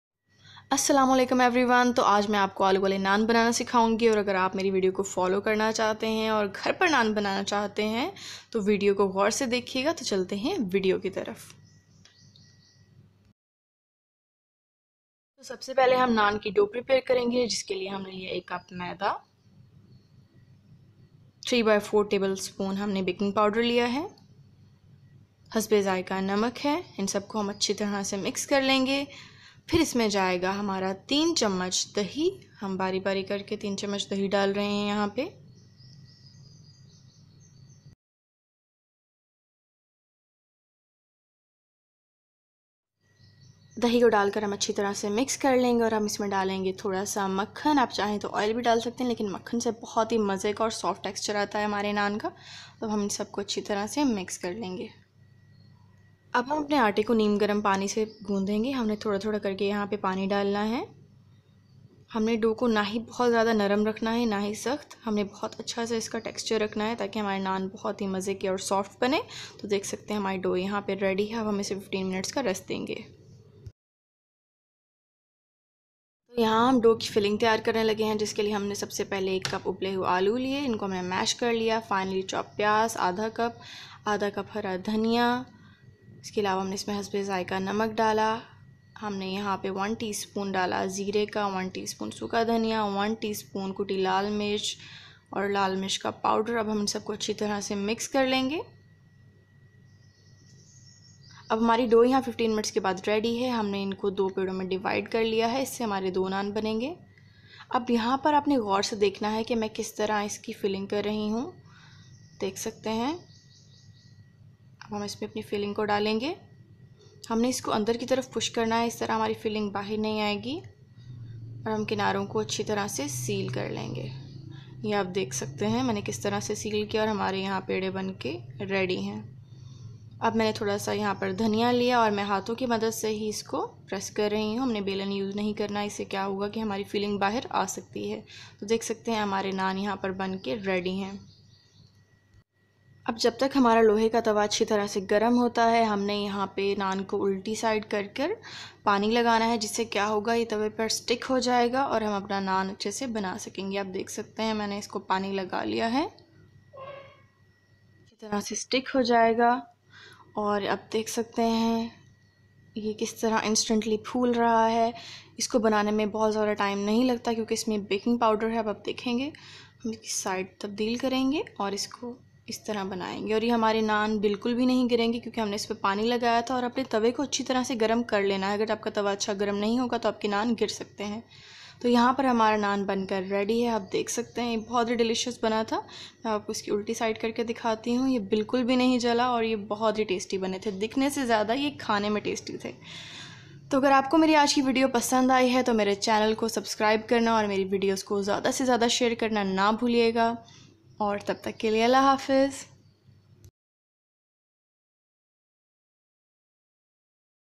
Aaloo walay naan on Tawa | No yeast | instant naan recipe | Lockdown Naan Recipe by Aasan Pakwan
Potato naan recipe made at home
Easy tandoori naan recipe without yeast
Instant Naan recipe